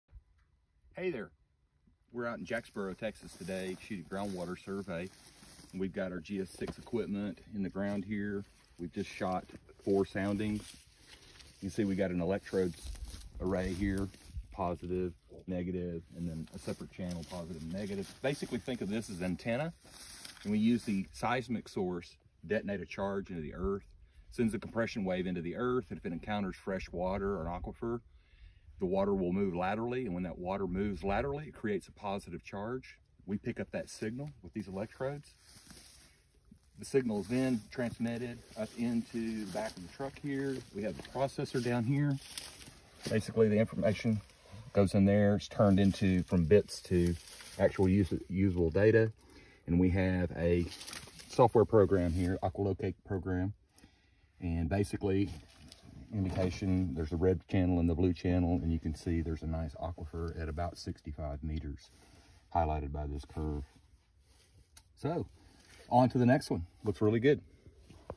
Finding groundwater aquifers using the Aqualocate GF6 seismic system, by American Water Surveyors.
Looking for groundwater?  Need a new well on your property?
This brief video offers an explanation of the system used by American Water Surveyors to find groundwater.  Go to WeFindWater.com to book your survey.